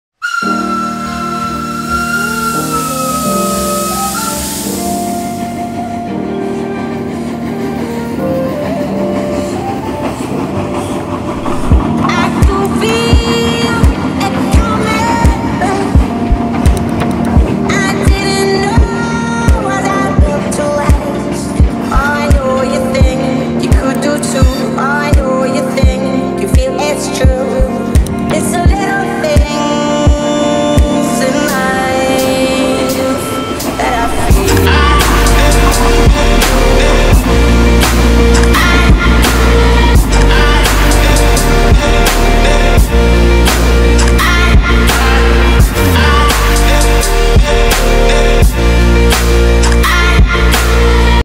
Puffing Billy
Puffing Billy making the journey from Gembrook to Belgrave over the trestle bridge in the Dandenong Ranges